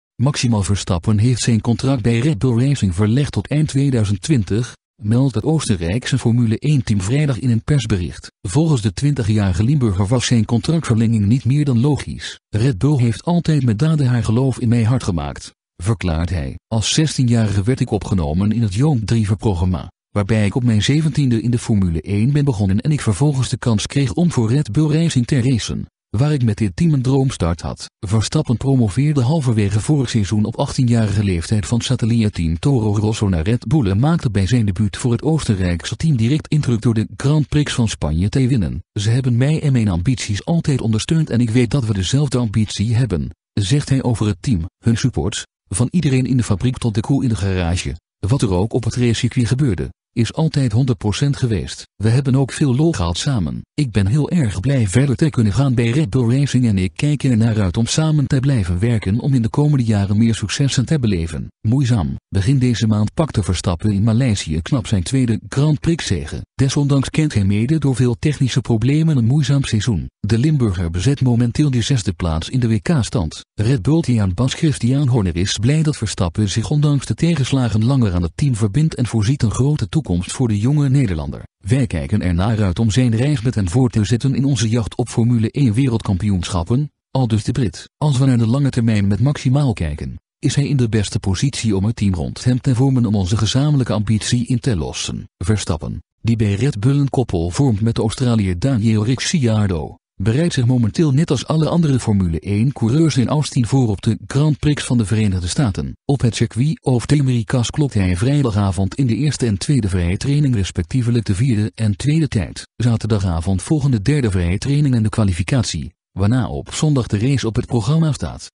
Max Verstappen blijft bij Red Bull Racing tot 2020
Max Verstappen heeft zijn contract bij Red Bull Racing verlengd tot eind 2020, meldt het Oostenrijkse Formule 1-team vrijdag in een persbericht.

Volgens de 20-jarige Limburger was zijn contractverlenging niet meer dan logisch. "Red Bull heeft altijd met daden haar geloof in mij hardgemaakt", verklaart hij.

"Als 16-jarige werd ik opgenomen in het young driverprogramma, waarna ik op mijn zeventiende in de Formule 1 ben begonnen en ik vervolgens de kans kreeg om voor Red Bull Racing te racen, waar ik met dit team een droomstart had."

Verstappen promoveerde halverwege vorig seizoen op 18-jarige leeftijd van satellietteam Toro Rosso naar Red Bull en maakte bij zijn debuut voor het Oostenrijkse team direct indruk door de Grand Prix van Spanje te winnen. "Ze hebben mij en mijn ambities altijd ondersteund en ik weet dat we dezelfde ambitie hebben", zegt hij over het team.

"Hun support, van iedereen in de fabriek tot de crew in de garage, wat er ook op het racecircuit gebeurde, is altijd honderd procent geweest. We hebben ook veel lol gehad samen. Ik ben heel erg blij verder te kunnen gaan bij Red Bull Racing en ik kijk ernaar uit om samen te blijven werken om in de komende jaren meer successen te beleven."
Moeizaam

Begin deze maand pakte Verstappen in Maleisië knap zijn tweede Grand Prix-zege. Desondanks kent hij mede door veel technische problemen een moeizaam seizoen. De Limburger bezet momenteel de zesde plaats in de WK-stand.

Red Bull-teambaas Christian Horner is blij dat Verstappen zich ondanks de tegenslagen langer aan het team verbindt en voorziet een grote toekomst voor de jonge Nederlander.

"Wij kijken ernaar uit om zijn reis met hem voort te zetten in onze jacht op Formule 1-wereldkampioenschappen", aldus de Brit. "Als we naar de lange termijn met Max kijken, is hij in de beste positie om een team rond hem te vormen om onze gezamenlijke ambitie in te lossen."

Verstappen, die bij Red Bull een koppel vormt met de Australiër Daniel Ricciardo, bereidt zich momenteel net als alle andere Formule 1-coureurs in Austin voor op de Grand Prix van de Verenigde Staten.

Op het Circuit of the Americas klokte hij vrijdagavond in de eerste en tweede vrije training respectievelijk de vierde en tweede tijd. Zaterdagavond volgen de derde vrije training en de kwalificatie, waarna op zondag de race op het programma staat.